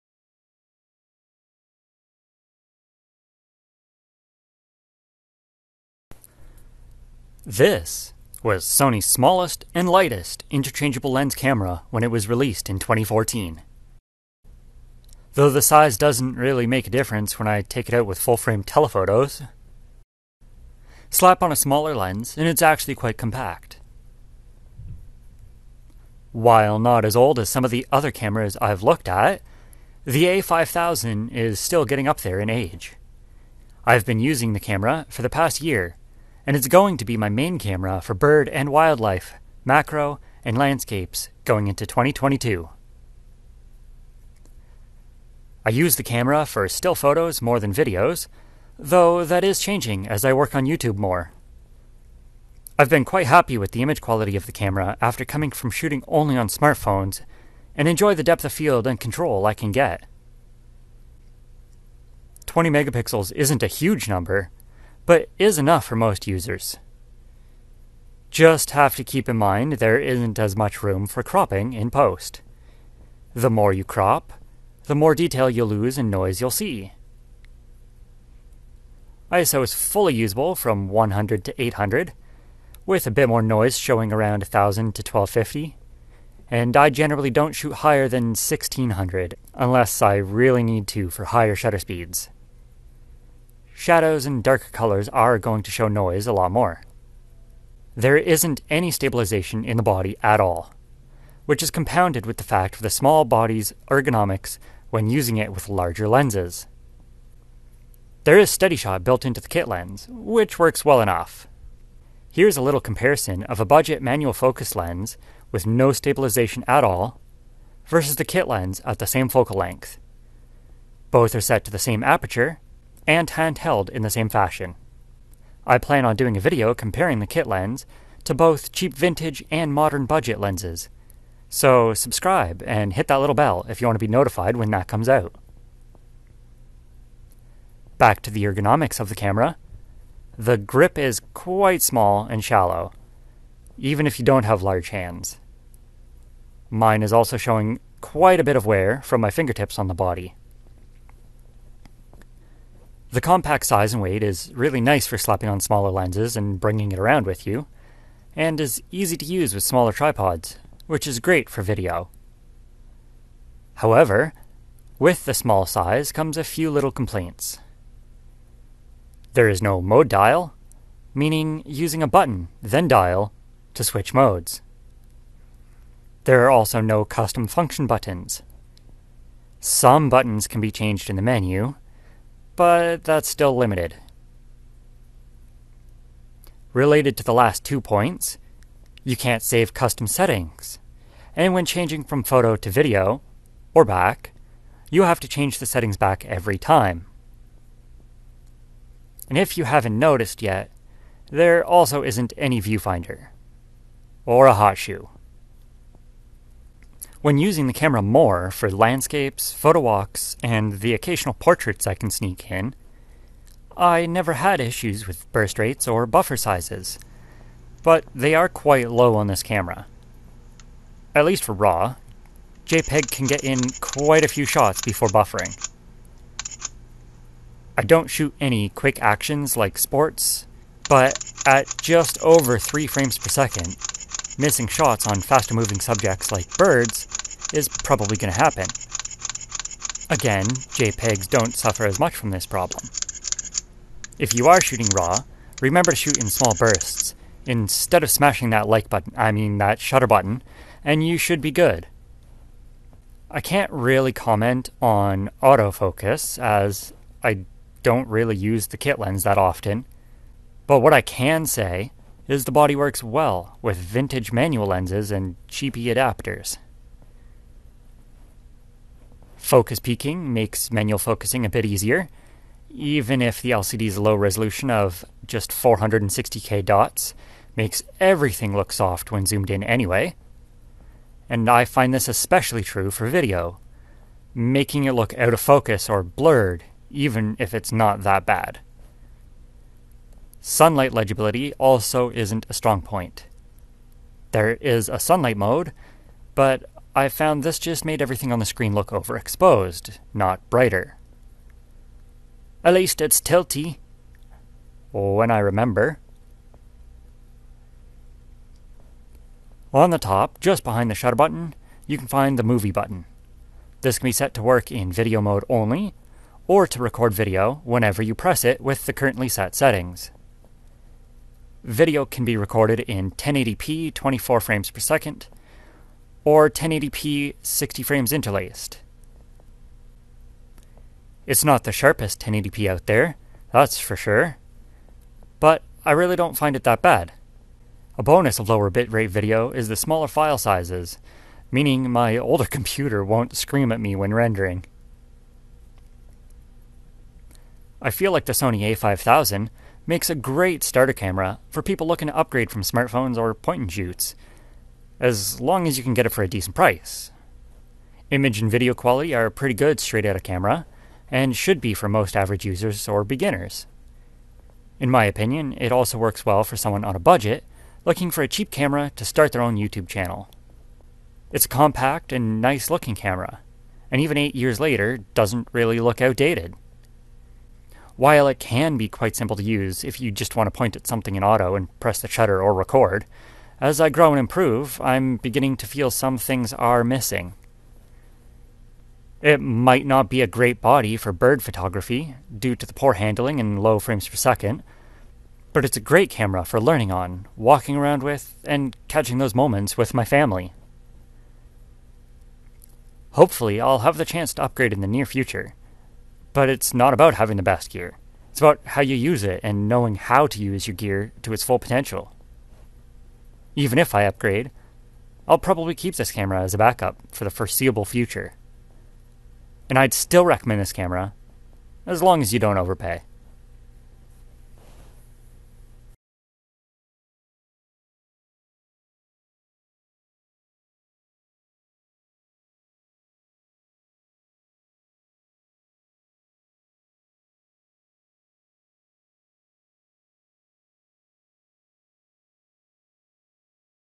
Sony A5000 in 2022?
I've used this camera throughout 2021, mainly for wildlife and macro, but also for landscapes and some portraits. It's far from a professional camera, and its definitely getting up there in age since it was released in 2014. But I'm going to keep using this as my main camera going into 2022 as I keep growing and improving, and going to try to get the most of the this body as I can.

Photo examples begin at 5:57

Lenses I use on this body:

Tokina SD 400mm f5.6 (MD mount)
Photax MC 80-205 f4.5 (OM mount)
Vivitar 28mm f2 Close focus (FD mount)
Neewer 32mm f1.6